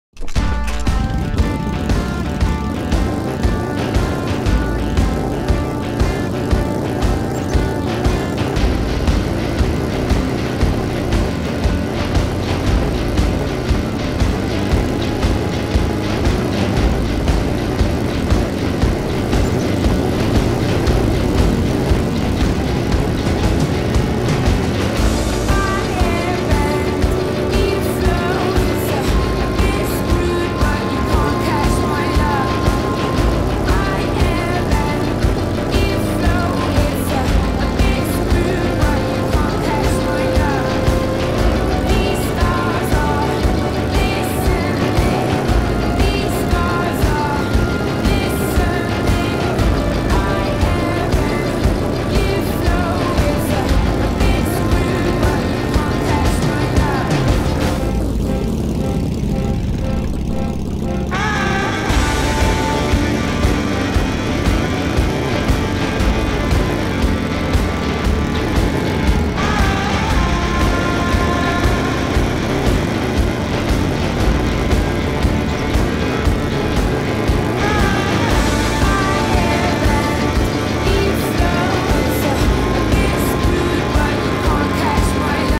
engine powered gokart
title says all- uses the same motor as my offroad truck. no transmission, doesn't need it, not even a clutch. goes 30mph or so, only rear wheel drive.